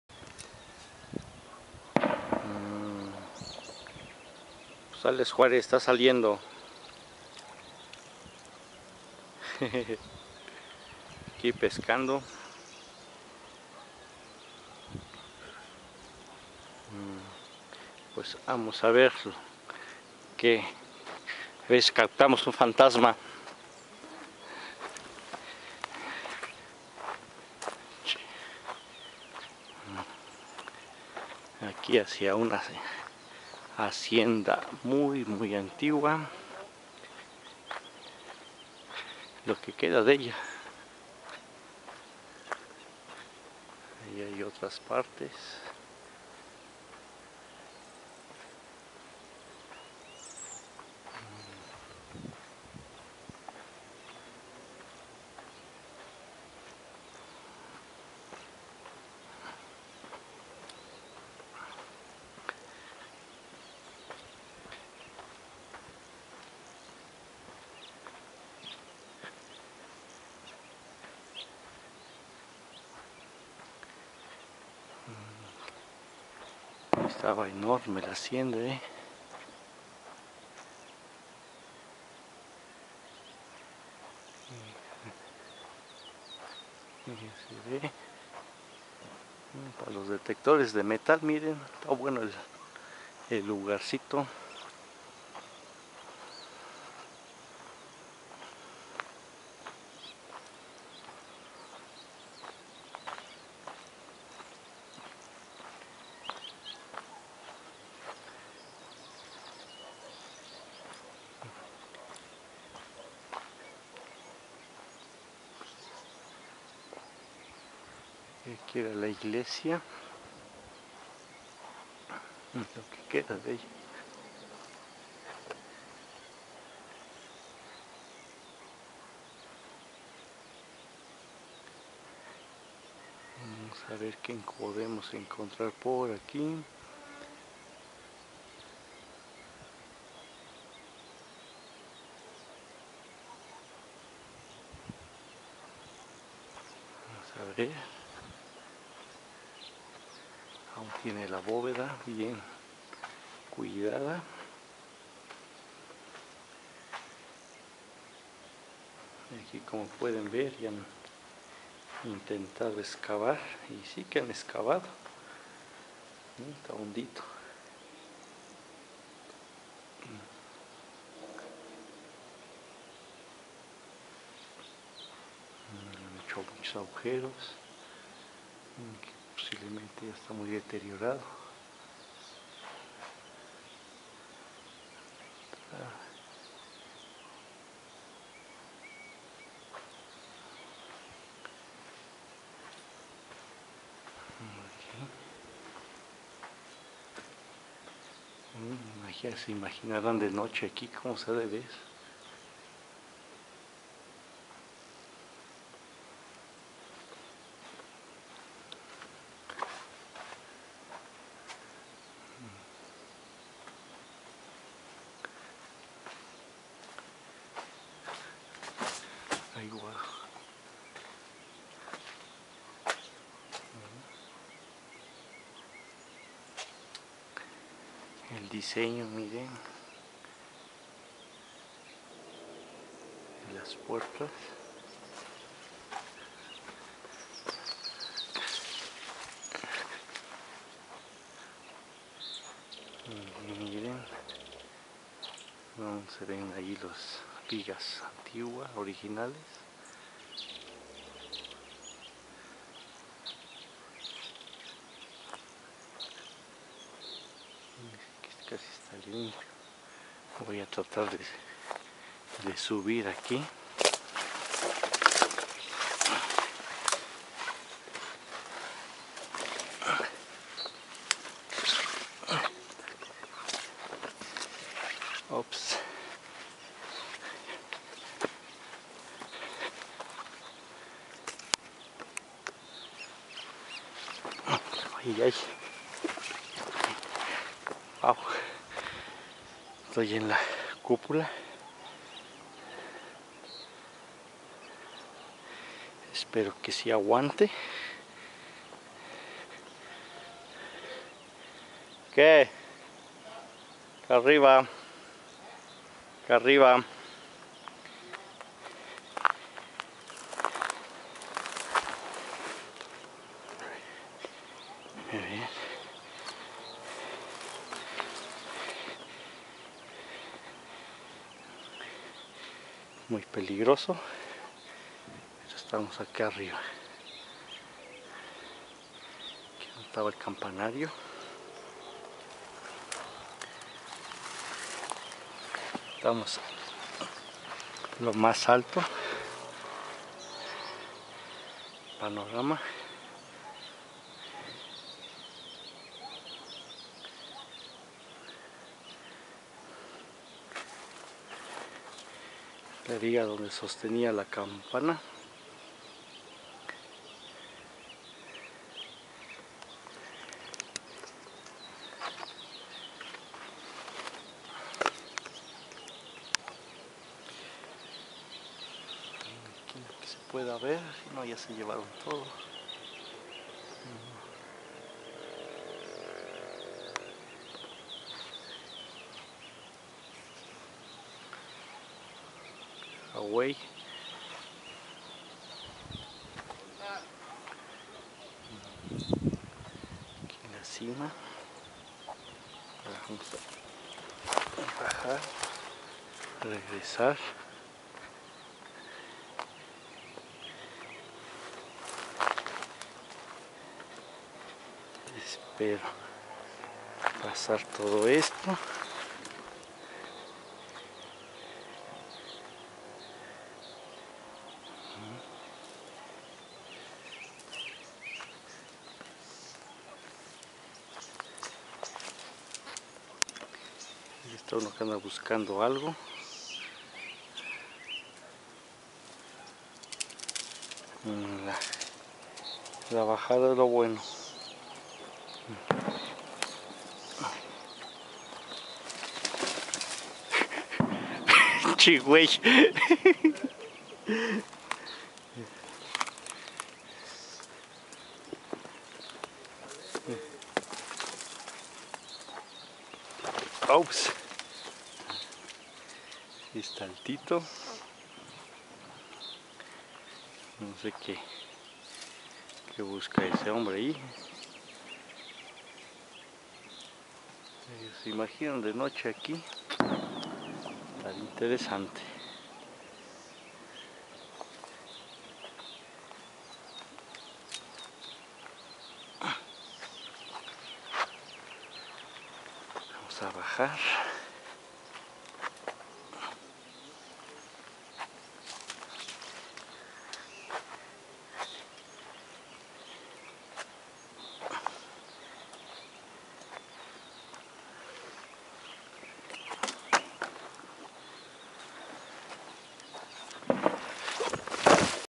Hacienda antigua para prospección
Aqui explorando una antigua hacienda parte 1/2